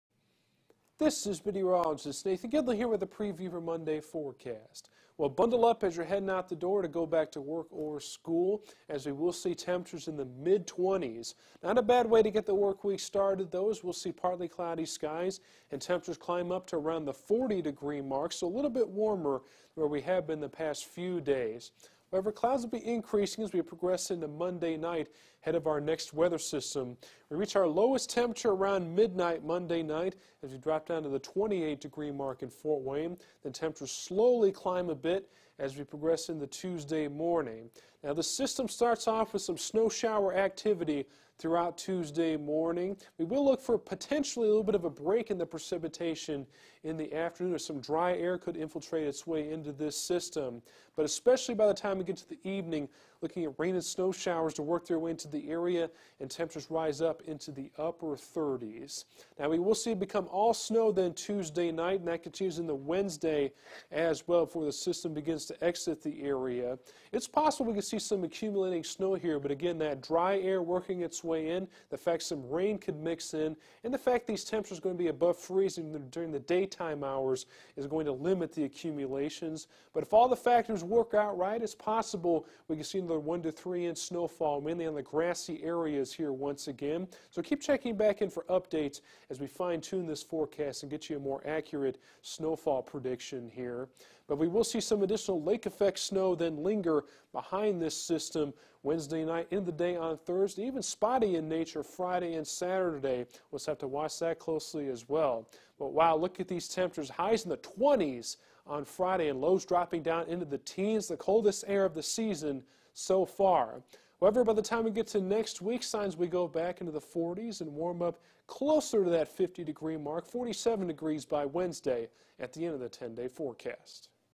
Monday Preview
A dry start to the workweek is on tap, with partly cloudy skies and highs reaching up to around the 40 degree mark.